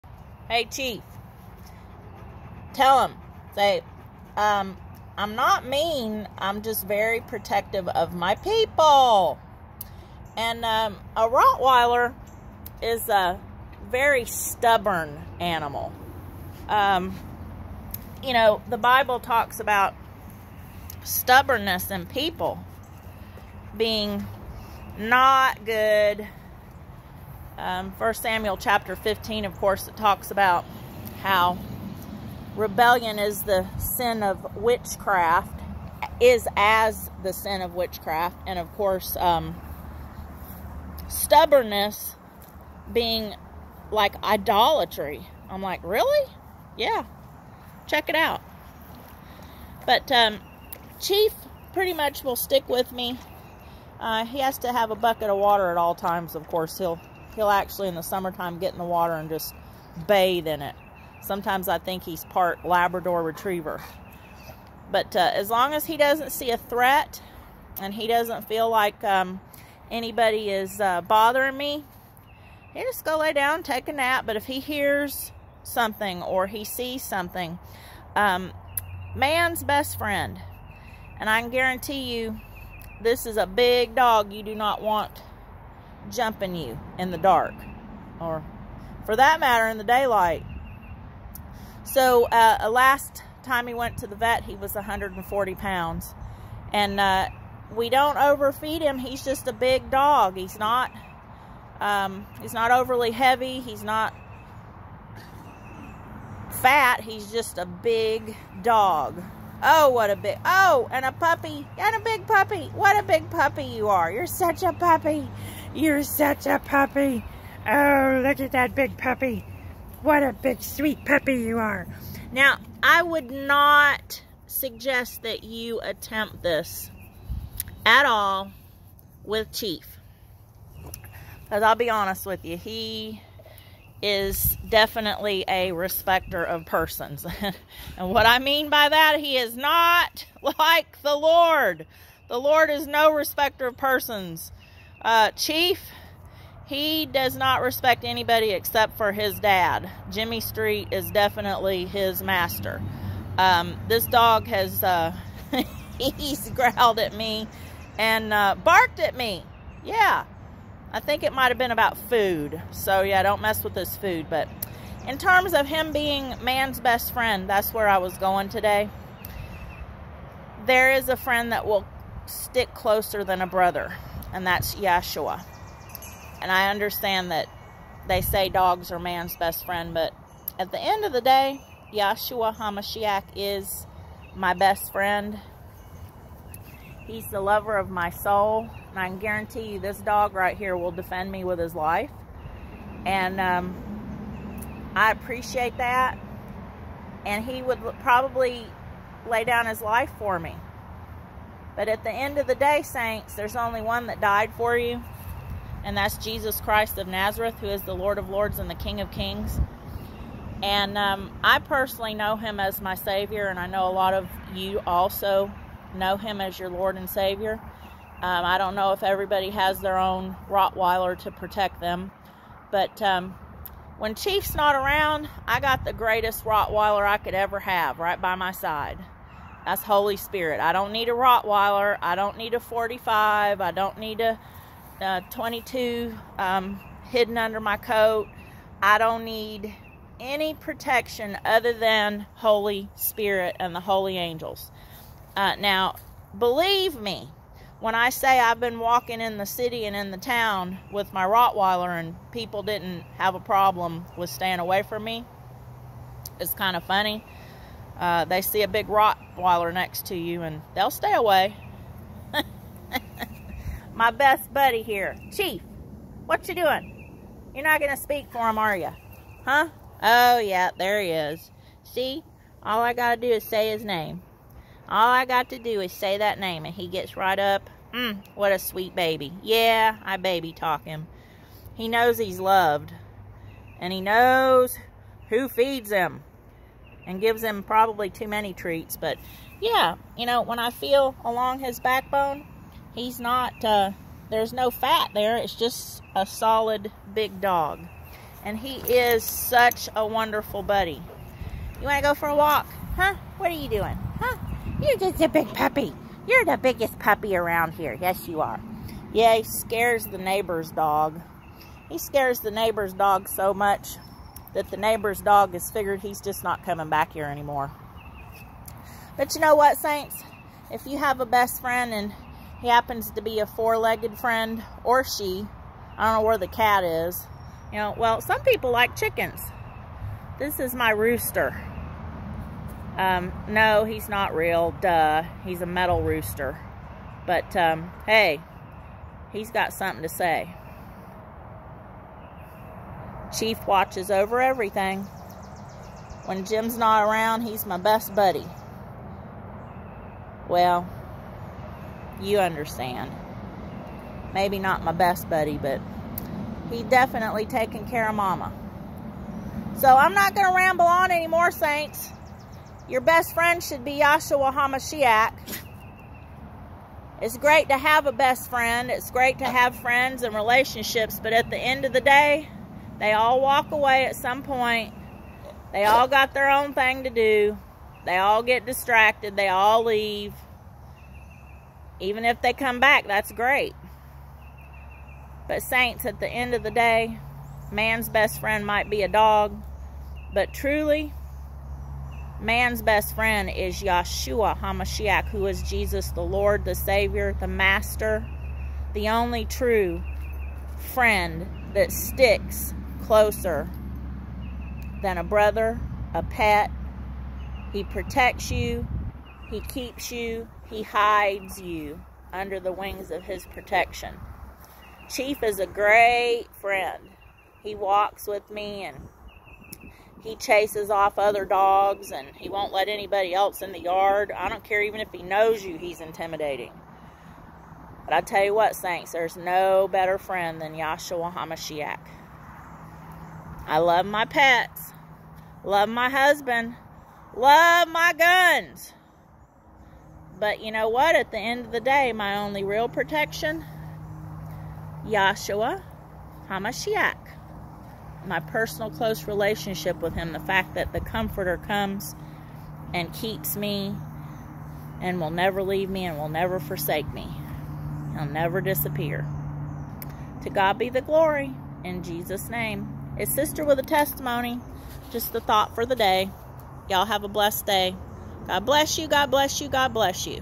CHIEF A ROTTWEILER: "REBELLION IS THE SIN OF WITCHCRAFT" 1 SAMUEL 15:23
Hey Chief, tell 'em: I'm not mean, I'm  just very protective of my people! And uh, a ROTTWEILER is  a very stubborn animal. Ya know the Bible talks about stubborness in people being not good! 1 Samuel 15 of course it talks about

"THE CHURCH W/O WALLS"

DEMONSTRATING THE KINGDOM OF HEAVEN ON EARTH